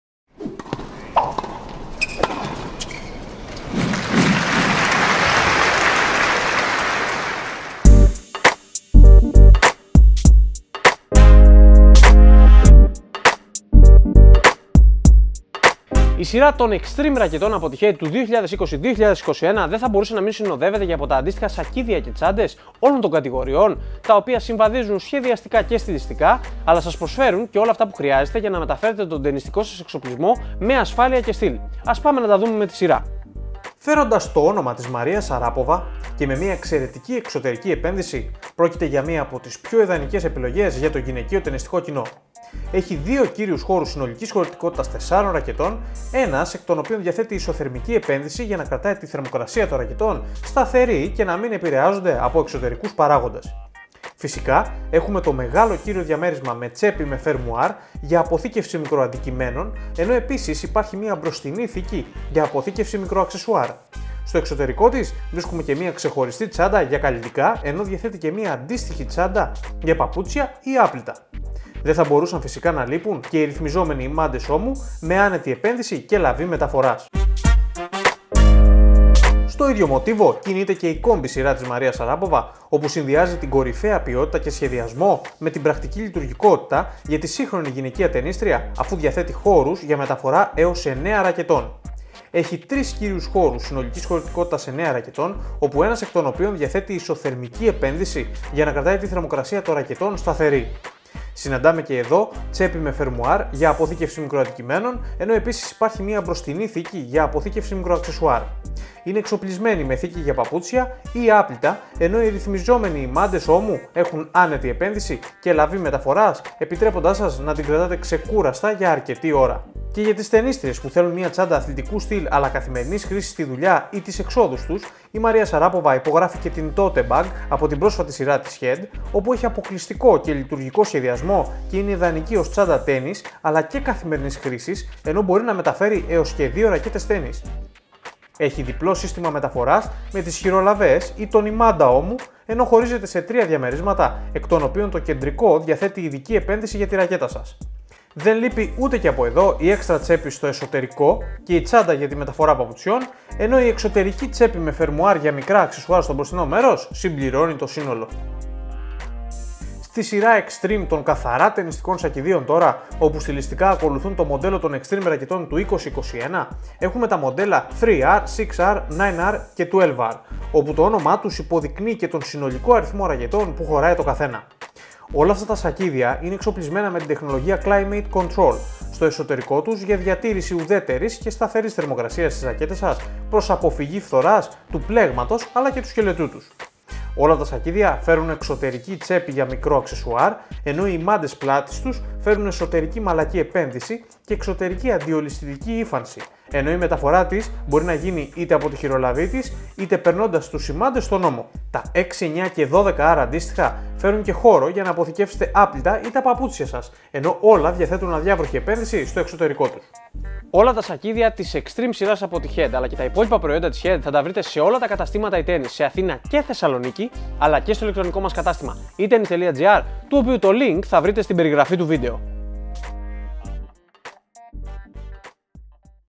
Τσάντες Τένις Head (Tennis Bags) review (GR)
Οι τσάντες τένις Head έχουν κορυφαία ποιότητα κατασκευής και μπορούν να καλύψουν τις ανάγκες τόσο του αρχάριου ερασιτέχνη παίκτη όσο και του κορυφαίου επαγγελματία.